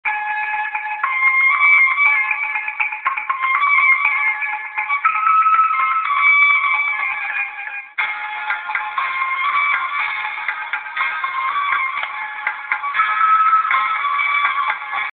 Song of Horse
Some douche bag thinks he can dance and make horses show up. (low quality vid-phone)